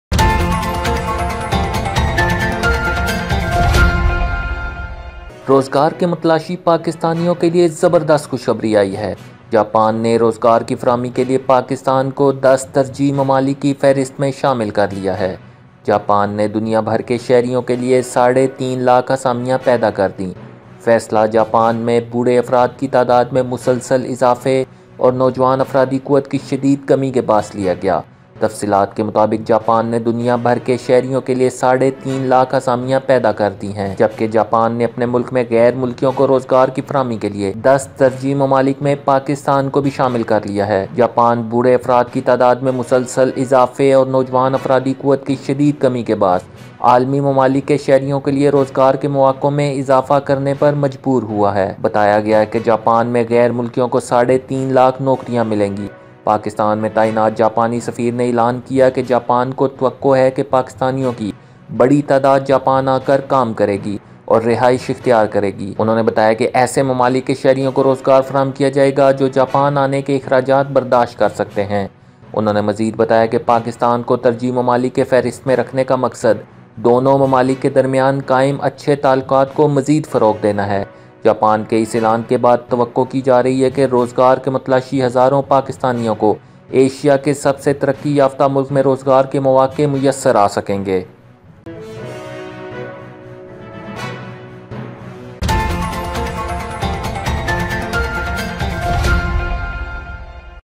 Japan Will Give Jobs To Pakistanis || Pakistan And Japan Relations
Japan has created nearly 3 million vacancies for citizens around the world. Japan has also included Pakistan in 10 priority countries for providing employment to foreigners in their country. Japan has been forced to increase employment opportunities for citizens of global countries due to the steadily increasing number of older people and the severe shortage of young workforce.
It has been reported that foreigners in Japan will receive three and a half million jobs. Japan's ambassador to Pakistan announced that Japan expects a large number of Pakistanis to come to Japan to work and take up residence. He said that jobs would be provided to citizens of countries that could afford the cost of coming to Japan.

Thankyou